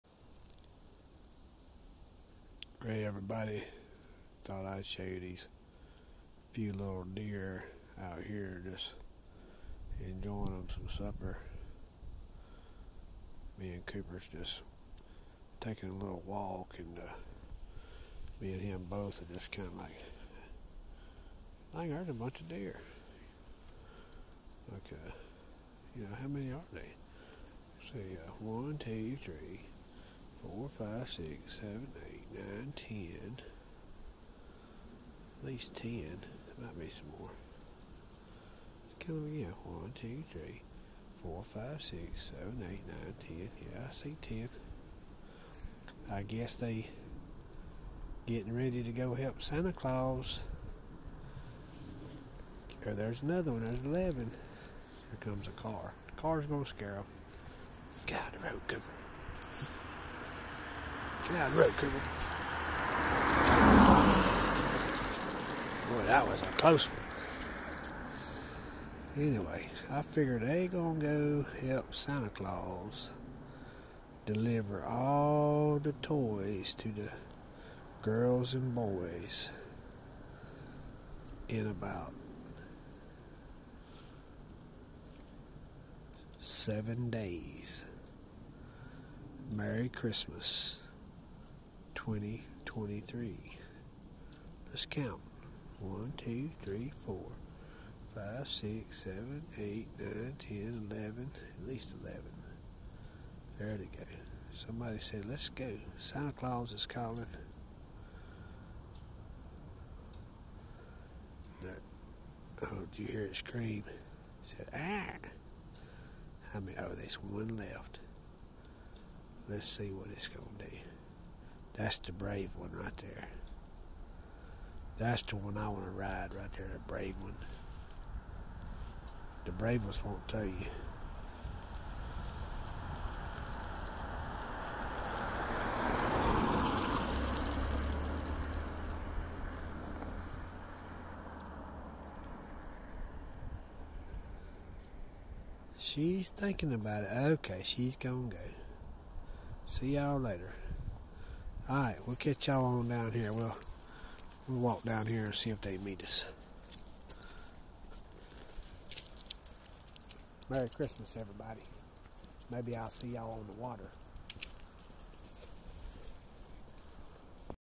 Santa's Christmas reindeer.
Reindeer are  on their way to help Santa Claus at Christmas time.